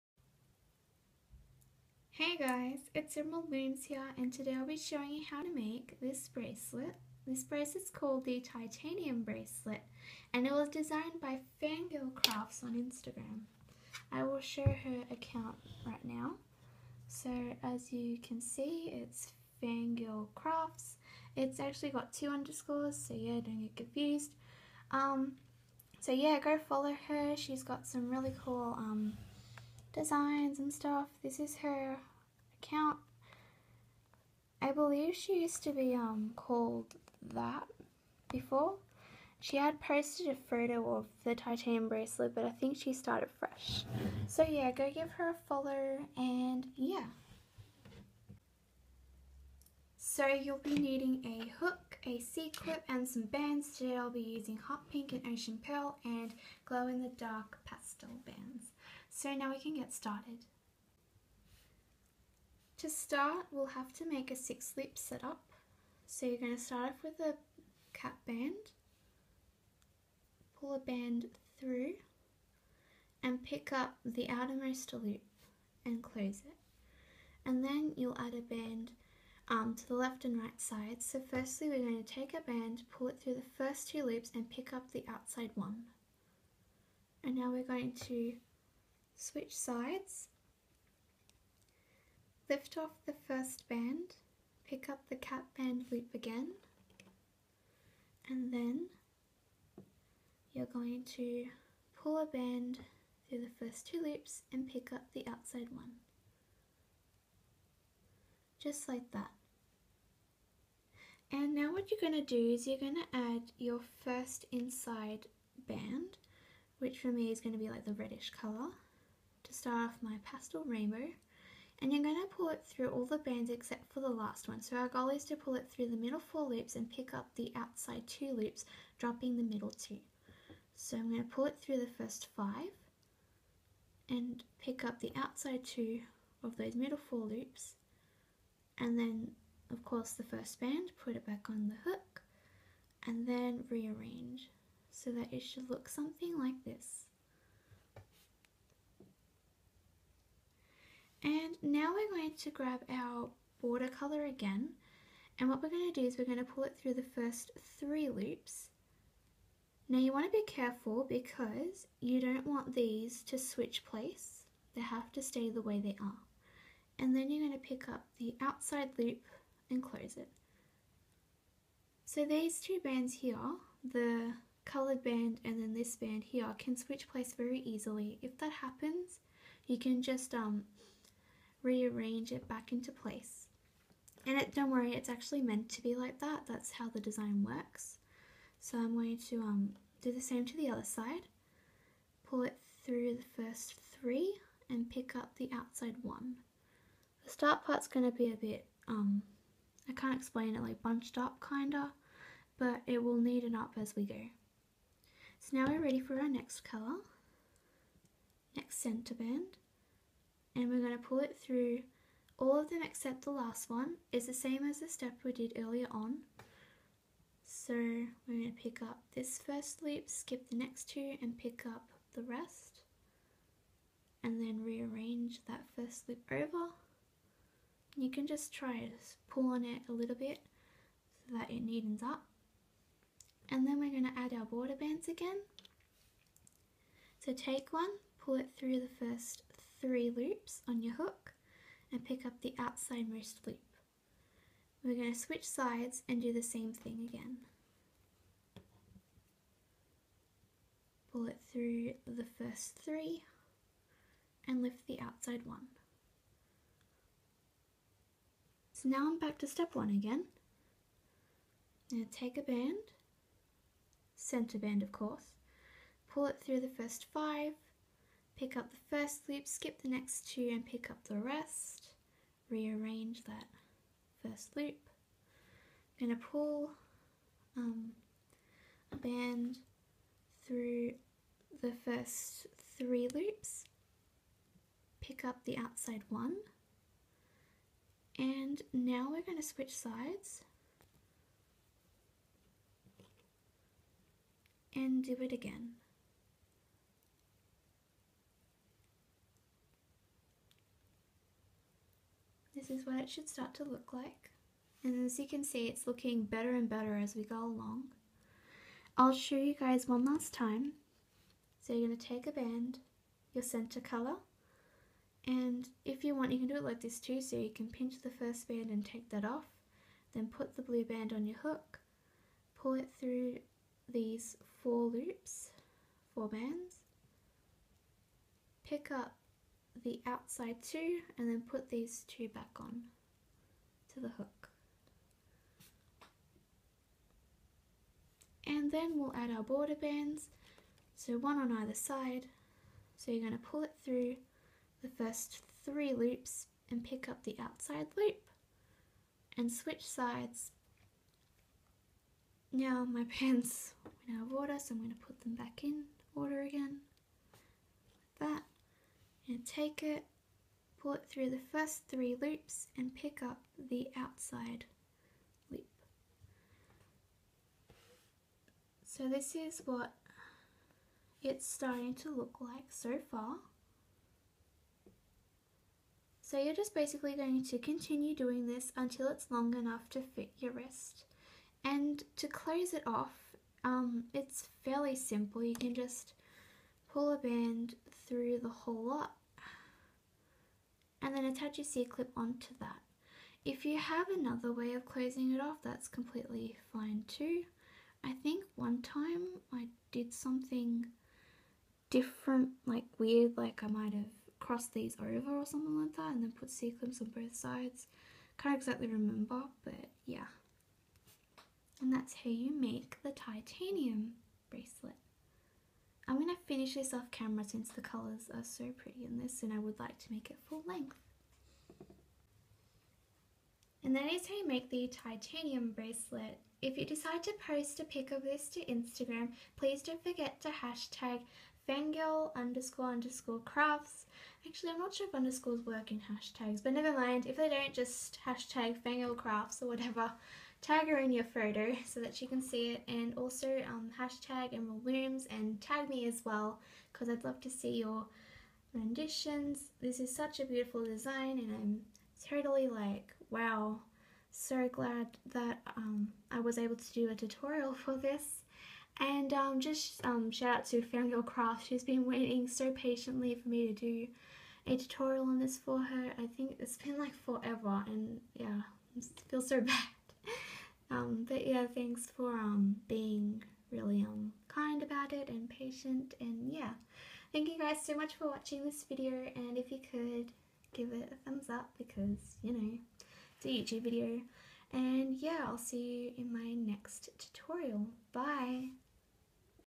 Titanium - Designed by @fangirl crafts | Hook Only Design | Rainbow Loom - How To
Hey guys!

In this tutorial I will be showing you how to make the Titanium bracelet designed by @fangirl__crafts on Instagram!

If you make this bracelet and decide to post it on Instagram, please don't forget to hashtag fangirl__crafts emeraldlooms and please do tag us in the pic!

~ Em